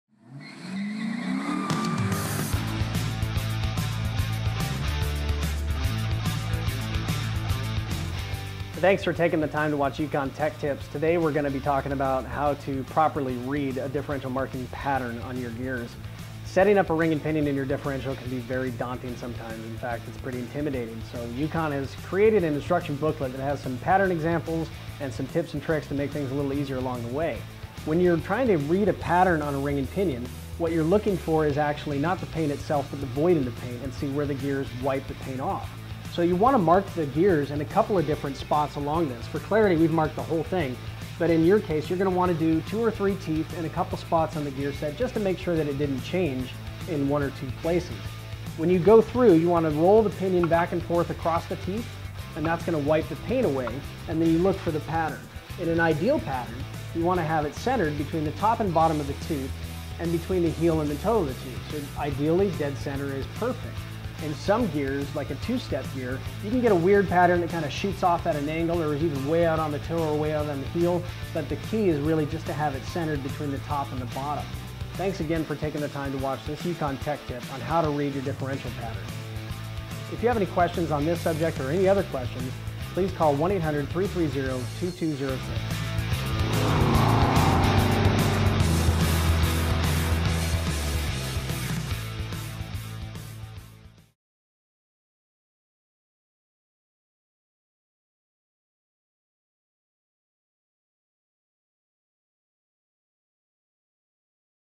Gear Tooth Contact Patterns Explained | Differential Tech Tips
One of the most common questions we're asked is how to properly read contact pattern markings during differential gear setup. In this video we give a quick demonstration of how to apply marking compound to the ring gear, and what to look for in the contact pattern on your gear teeth.